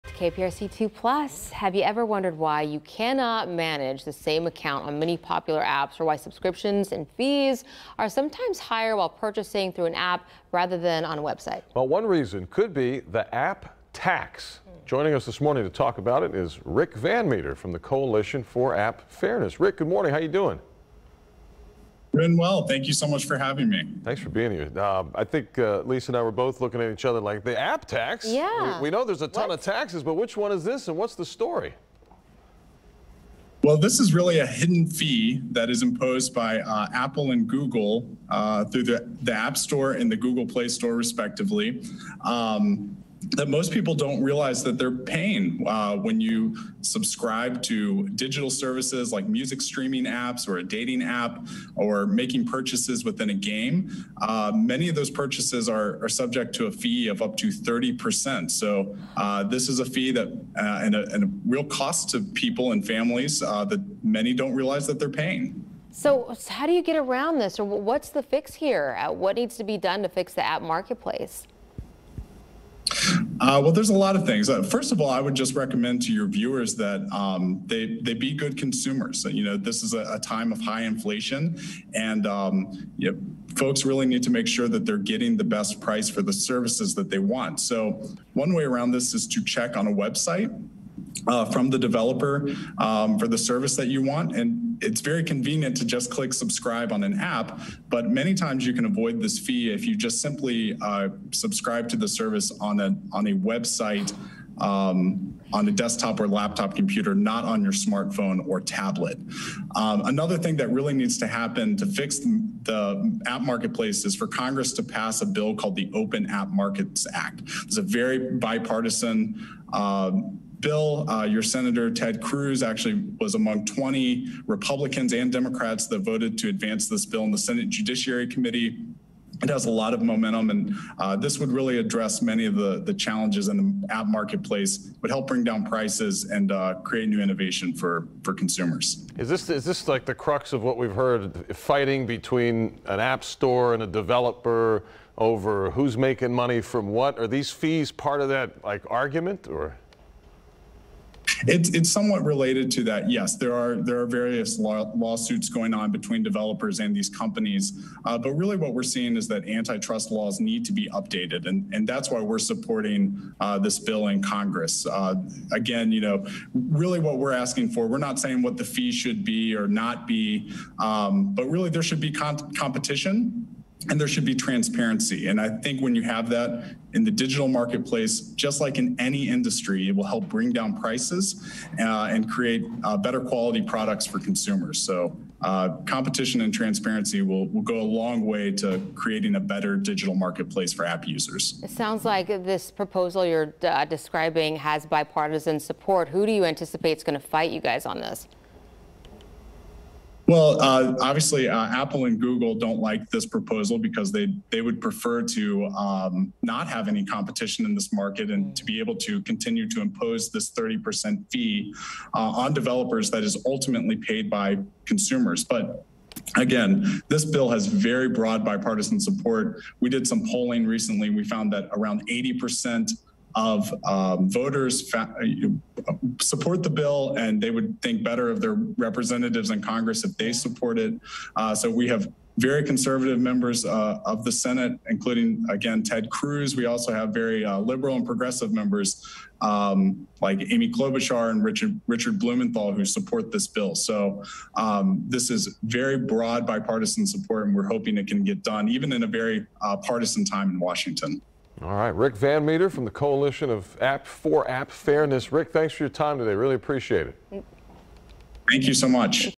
What you need to know when making in-app purchases
Have you ever wondered why can’t manage your account on many popular apps, or why subscriptions and fees are greater while purchasing through an app than on the website? One reason -- The “App Tax.”

Rick VanMeter of the Coalition for App Fairness appeared on KPRC 2+ Thursday to share more about this tax.